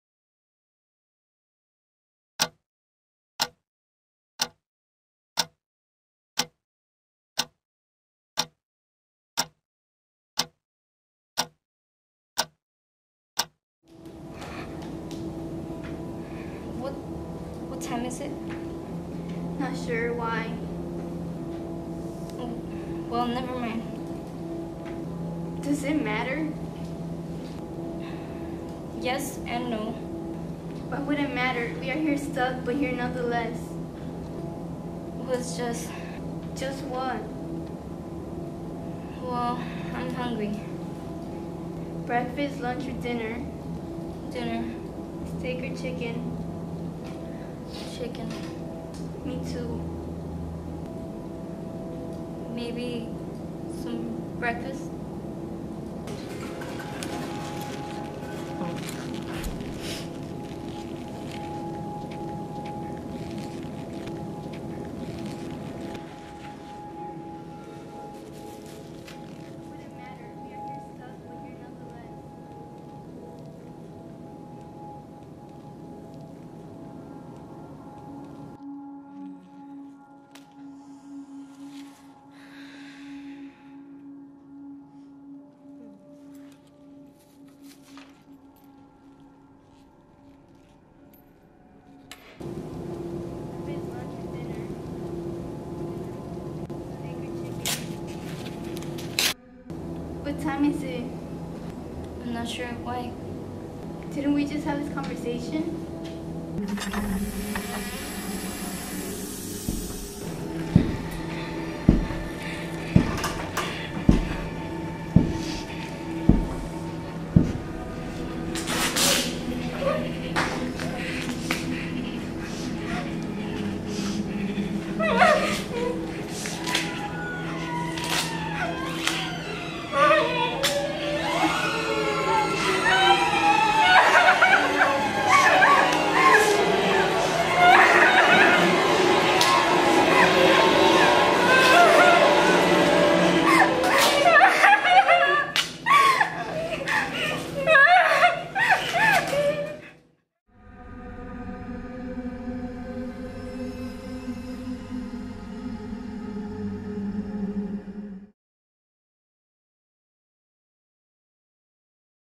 Captives - Short Film
A student short film based off a script called 'Steak or Chicken' for our media class.